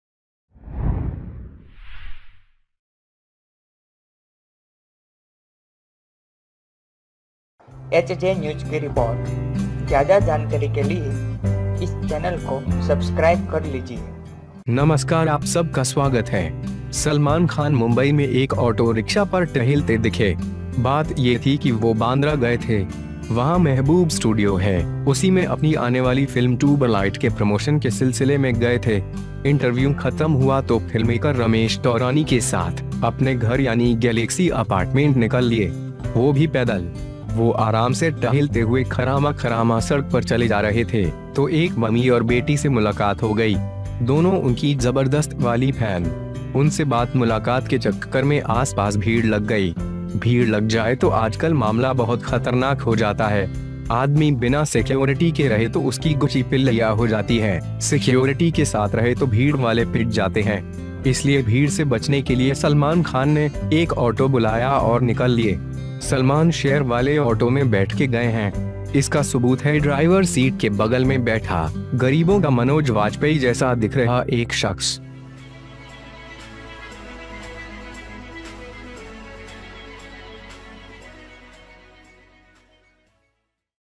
OMG ! ऑटो से सड़क पर निकले सलमान, चला कौन रहा था पता नहीं...
ऑटो से सड़क पर निकले सलमान, चला कौन रहा था पता नहीं...

 Salman Khan appears to be on a auto rickshaw in Mumbai. The thing was that he went to Bandra. There is Mahabob studio, in the same way, in connection with the promotion of his upcoming film Tubelight. If the interview ended, then with the filmmaker Ramesh Taurani, to get away from home, ie, the Galaxy apartment. They also walk on foot. They were going to the Khairama Kharama road on the road comfortably, and a mother and daughter met. Both their awesome fan. They talked to the crowd around the round about the meeting.

If there is a crowd, then the case becomes very dangerous nowadays. If the man is without security, his gucci gets squeezed, if he is with the security, then the crowd gets beaten. So to avoid the crowd, Salman Khan called an auto and got out.

 Salman shares have been seated in the auto. The evidence is that the driver sitting next to the seat, a man resembling Manoj Bajpei of the poor.